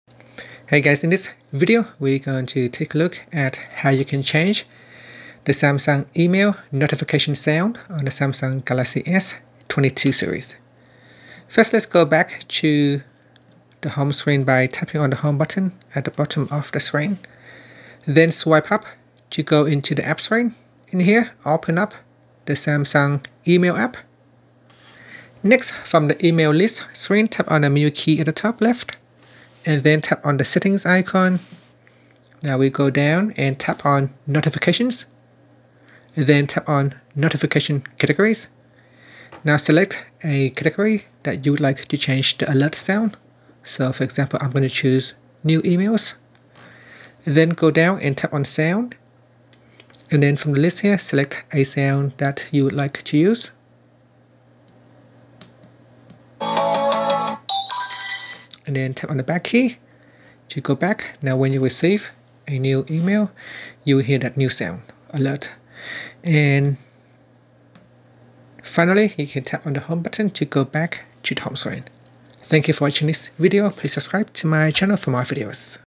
Galaxy S22/S22+/Ultra: How to Change Samsung Email Notification Sound
Learn how you can change Samsung Email Notification Sound on the Samsung Galaxy S22/S22+/Ultra.